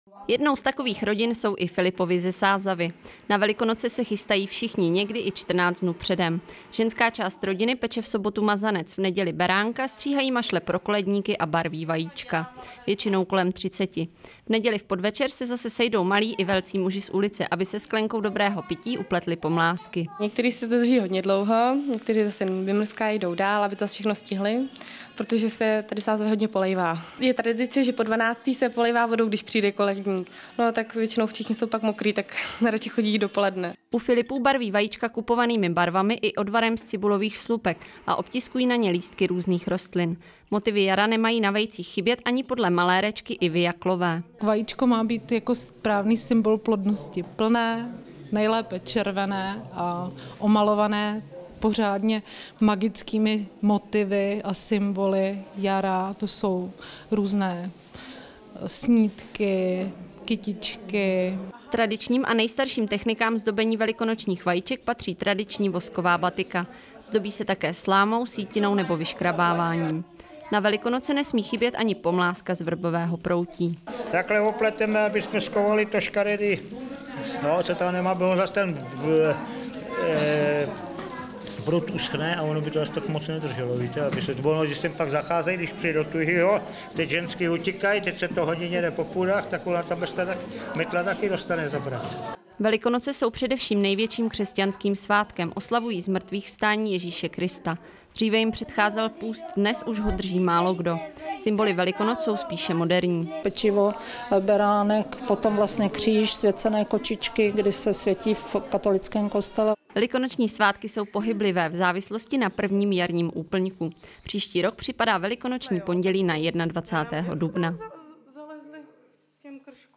Velikonoční zvyky na Žďársku stále přežívají
Vysočina/Sázava - Přestože to při pohledu na záplavu sněhu nevypadá (nevypadalo), Velikonoce jsou za námi. Přestože doba je uspěchaná a tradicím moc nepřeje, hlavně na vesnicích se ještě najde hodně rodin, které lidové velikonoční zvyky dodržují.